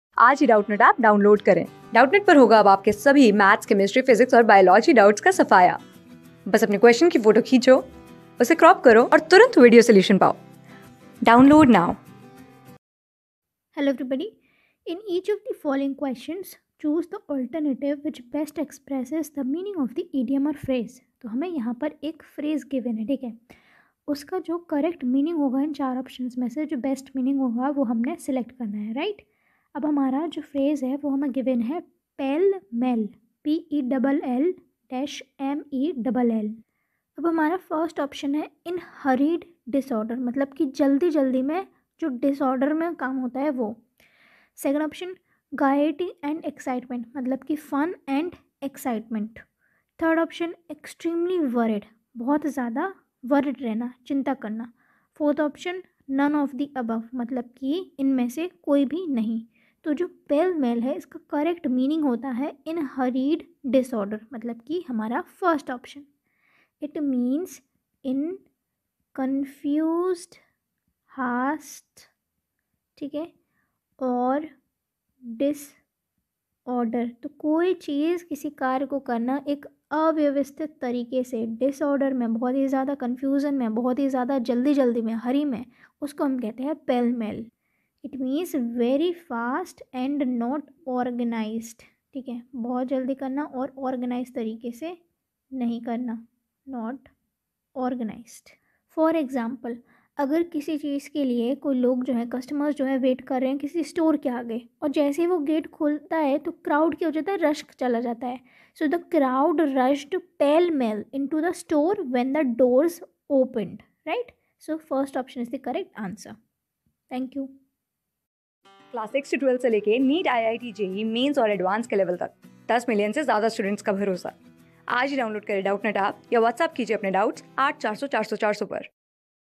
In each of the following questions, choose the alternative which best expresses the meaning of t...
In each of the following questions, choose the alternative which best expresses the meaning of the idiom/phrase. Pell-mell
Class: 14
Subject: ENGLISH
Chapter: IDIOMS AND PHRASES
Board:GOVT

👉 Have Any Query? Ask Us.